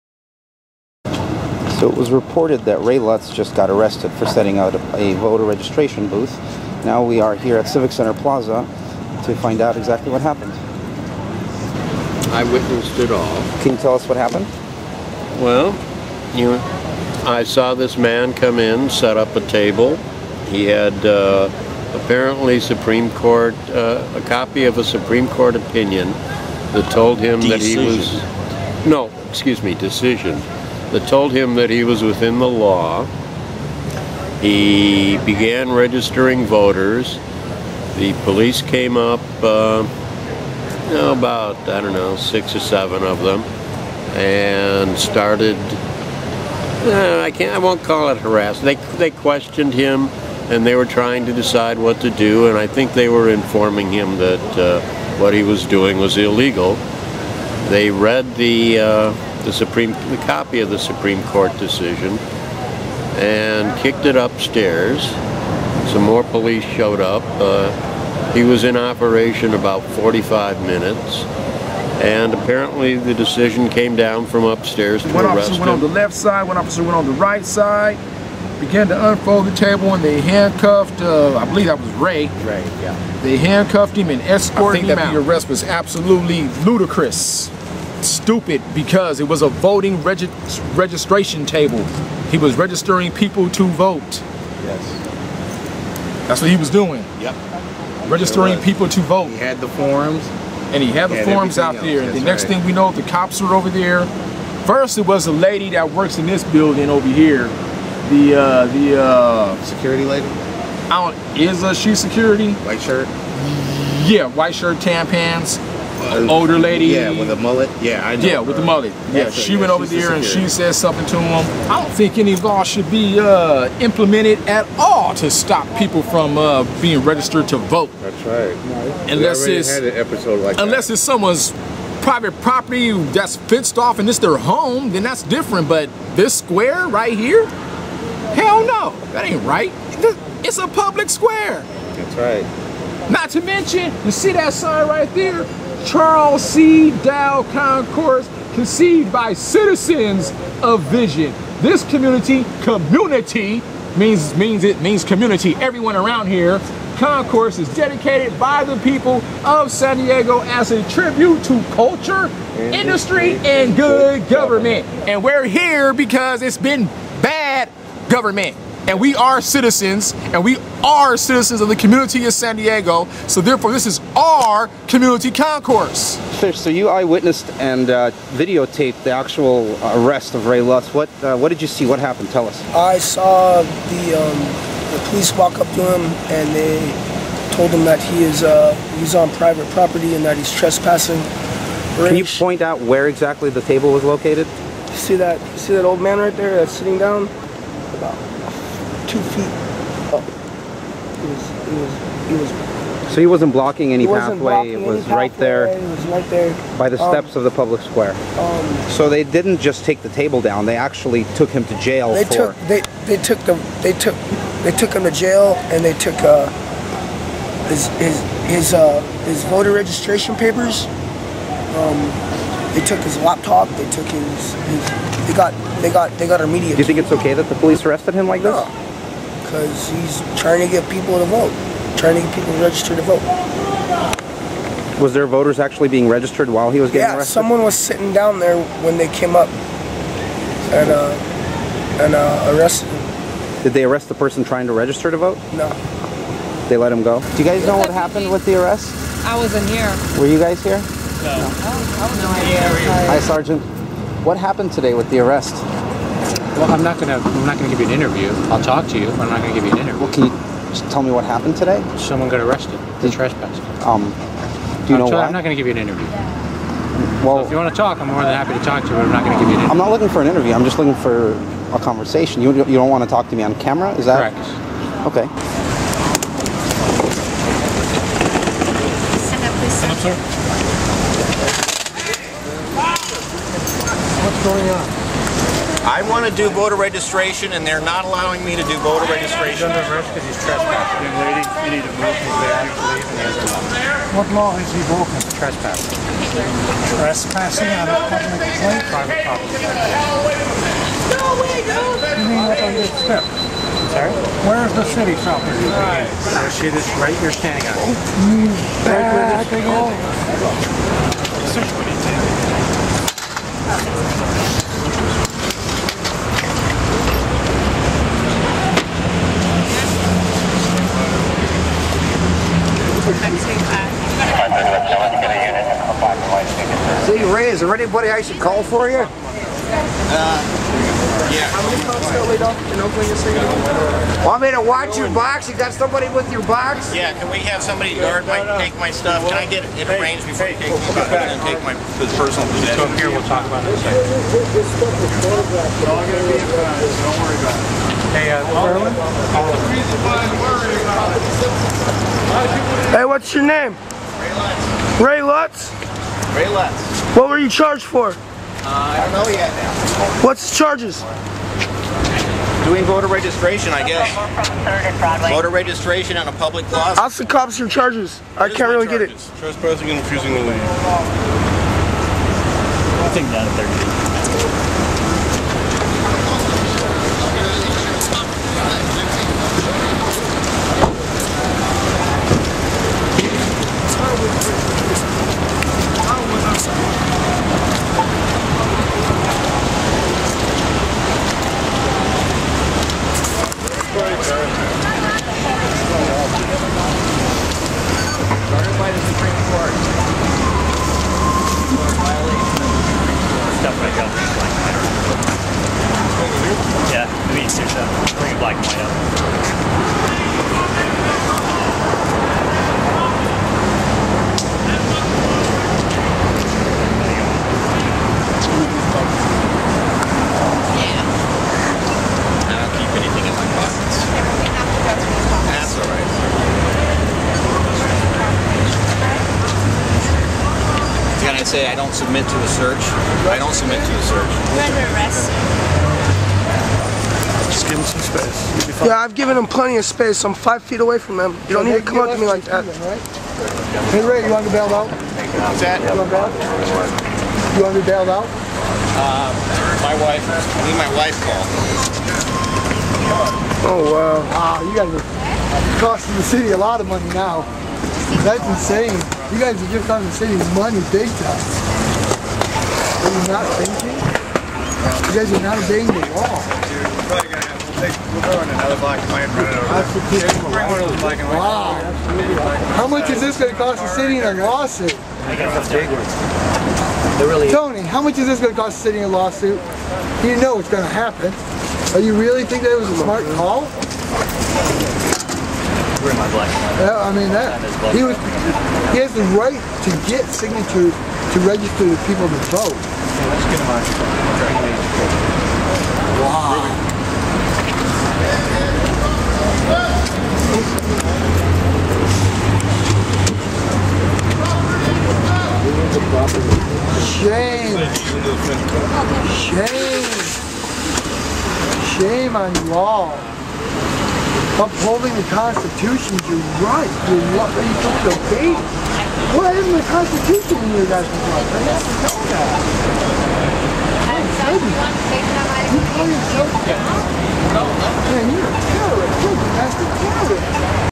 Occupy SD Ray Lutz Arrested for Registering Voters Nov 29
Ray Lutz, a community organizer and former congressional candidate was arrested at Civic Center Plaza. This video shows interviews with the eye witnesses, police, as well as the actual footage of the arrest.